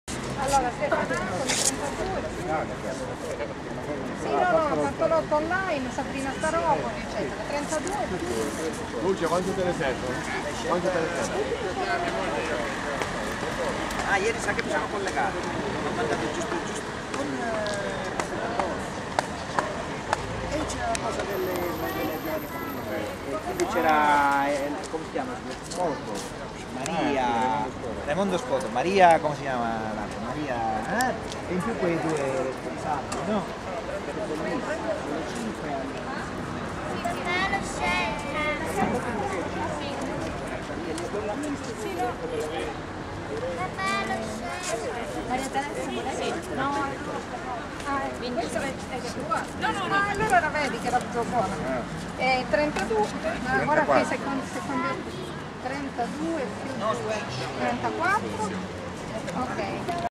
Azalea della Ricerca - Vibo Valentia
Il momento in cui ci hanno consegnato le azalee a Vibo Valentia